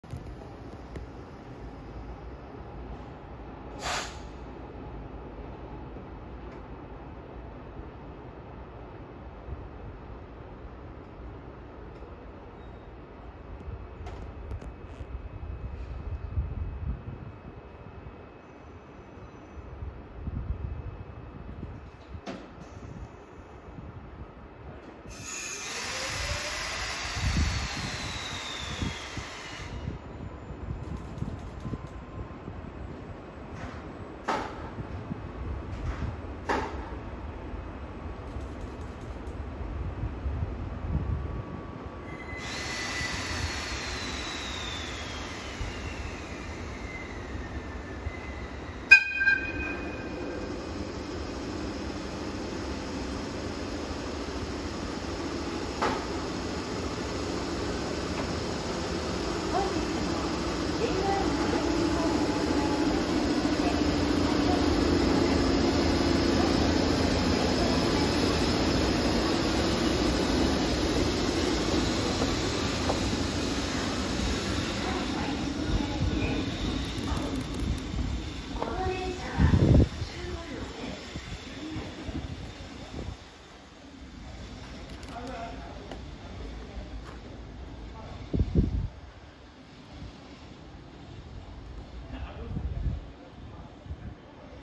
【新小岩操車場内機回し・入れ替え作業の様子】国鉄EF65形2000番台2068号機（赤プレート・国鉄色）が構内をブロワー起動しノッチ試験し警笛鳴らし発車するシーン（5971レ牽引機）2022.6.11
またYouTubeの不具合で最初変なタイトルで上がったよ。

今のところ毎日見れるらしいです。
近い未来貴重になりそう…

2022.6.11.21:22